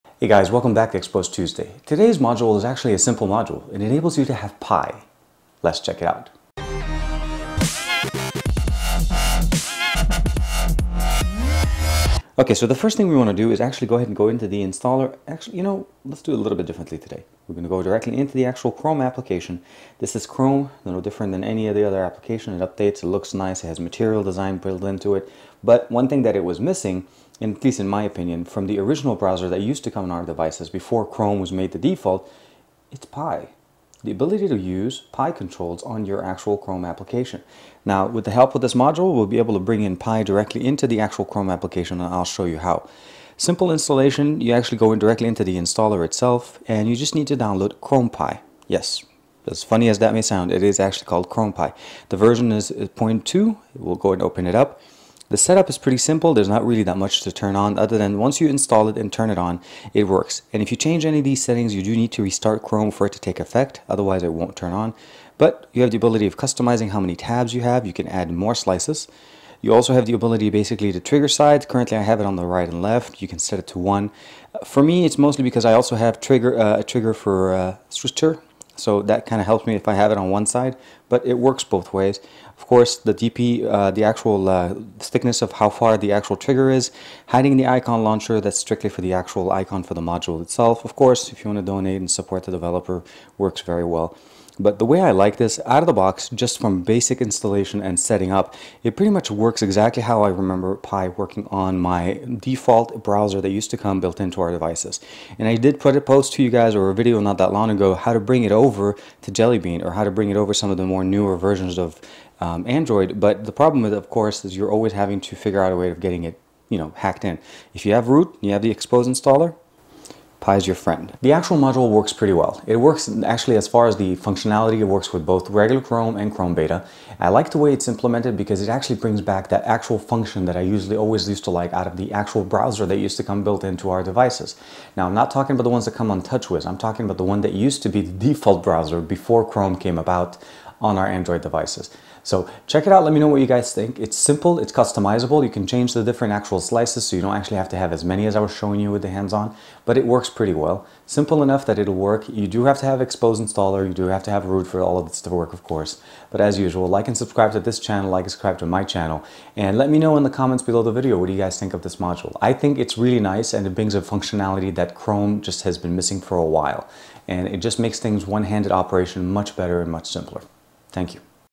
Get Some Pie for Your Chrome- XDA Xposed Tuesday
Chrome has become Android’s default browser. As great as Chrome is, it’s still missing some ease of use features. Sometimes on our phablet devices we need two hands to work in Chrome, or you can do the awkward thumb stretch. So simple Pie Controls would help with the navigation.

In this episode of XDA Xposed Tuesday, XDA TV Producer TK reviews an Xposed Module that lets you enable Pie Controls in your Chrome browser. XDA Forum Member JT5 created the aptly named ChromePie module. TK shows off the modules and gives his thoughts, so check out this Xposed Tuesday video.